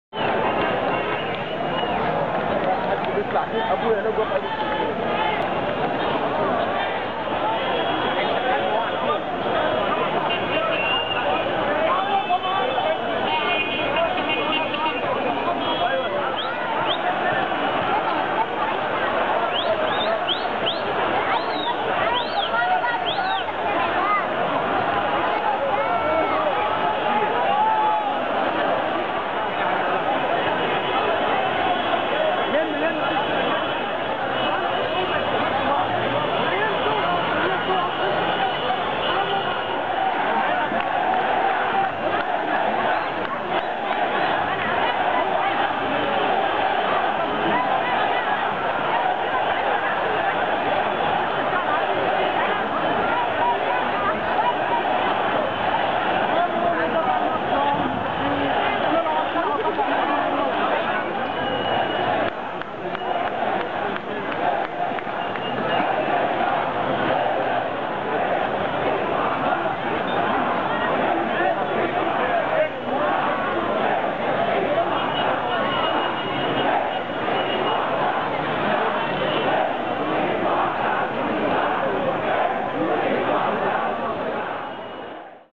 Clashes at Tahrir Square Friday 12.8.2011
Riot and military police are occupying Tahrir square while protesters are ralliying in favour of a civil state. Suddenly the protests turned violent. Protesters started throwing rocks after military police tried violently to disperse a crowd of protesters then military police threw stones back.

It was only a short incident.